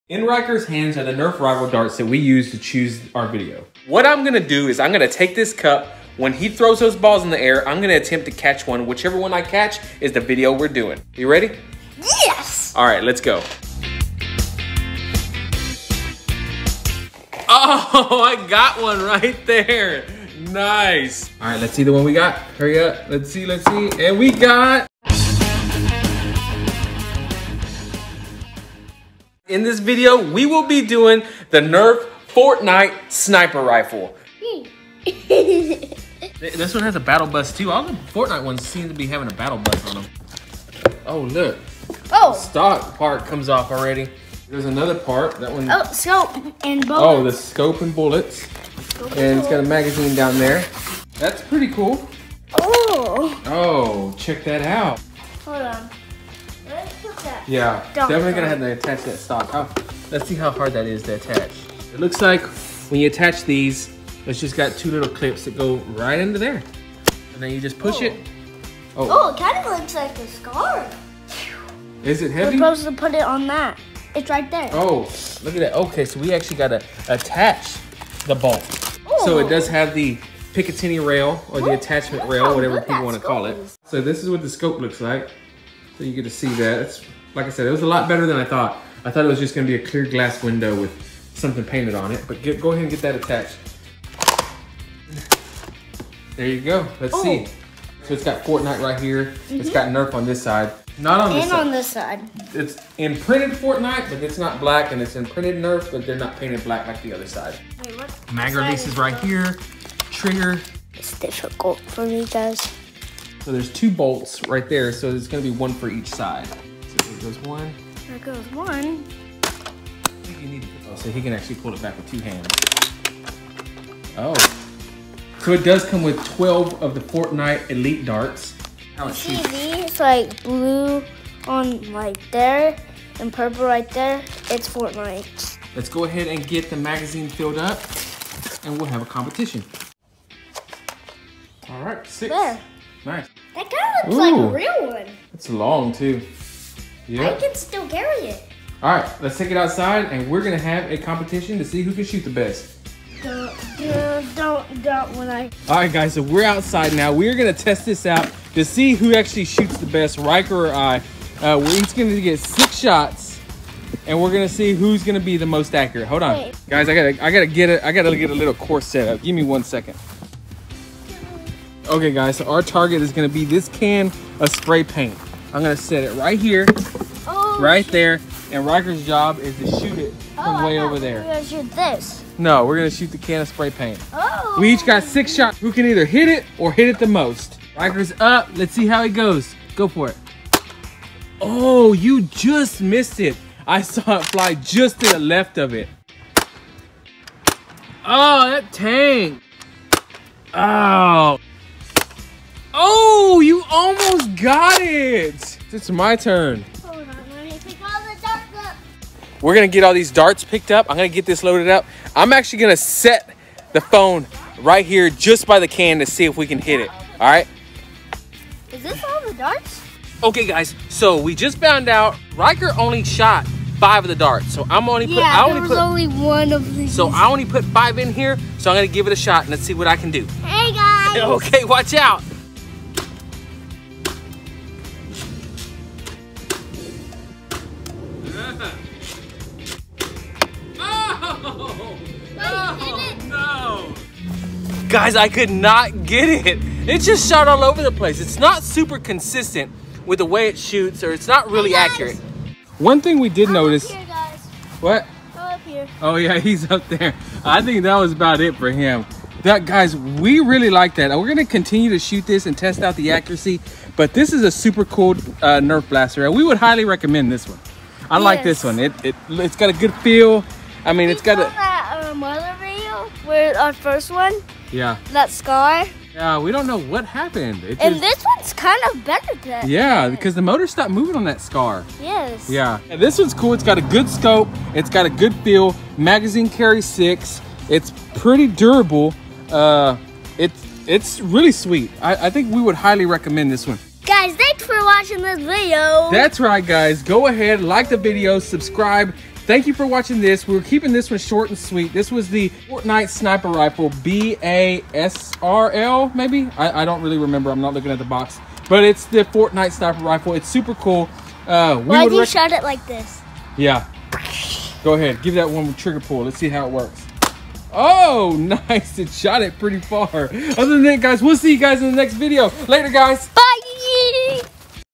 The Fortnite Sniper Rifle Nerf Gun
Today we unbox the Fortnite Sniper Rifle Nerf Gun called the BASR-L. this Nerf gun is massive and looks just like the fortnite sniper rifle. lets test out the accuacy to see if its as powerful as the fortnite game version.